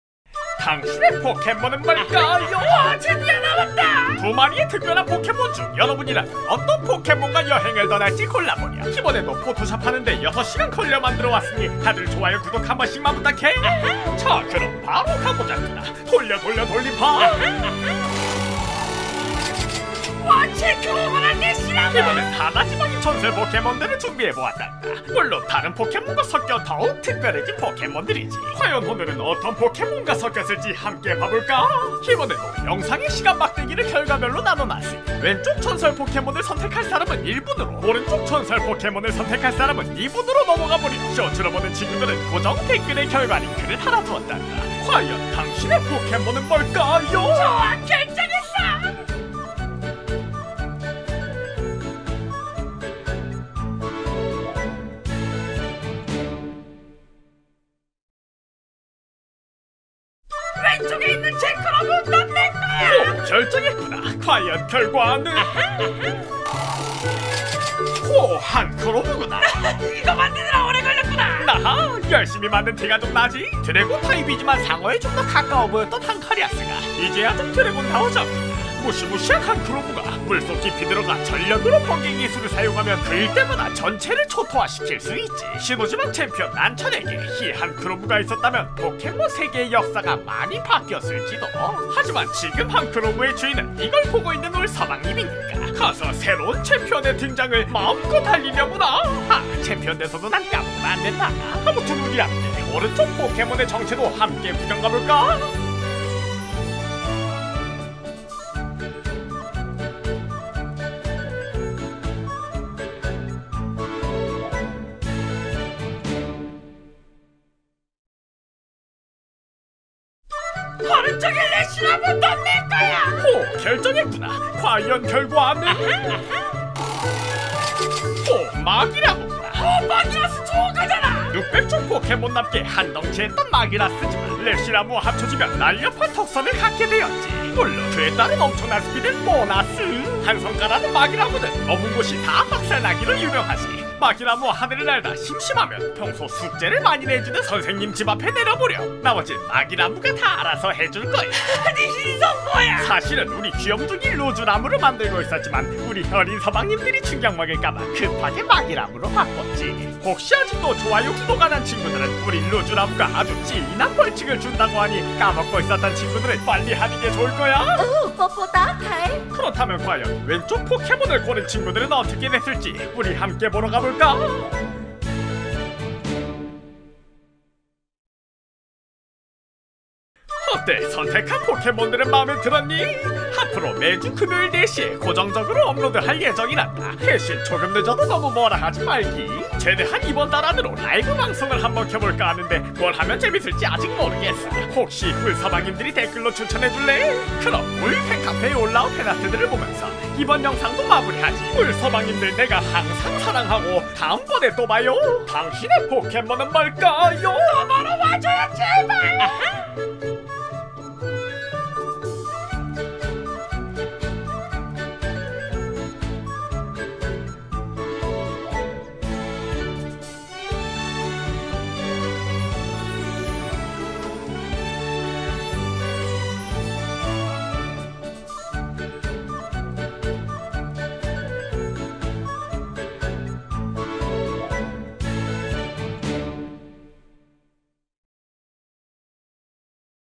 스포) 이 중 하난 한카리아스와 합체합니다. 제크로무🎇 vs 레시라무✨ 당신이라면 어떤 포켓몬을? [병맛더빙] | 당신의 포켓몬은 뭘까요? 6탄
0:00 당신의 포켓몬은 뭘까요?
1:00 왼쪽 포켓몬!
2:00 오른쪽 포켓몬!
3:00 방송공지, 팬아트자랑!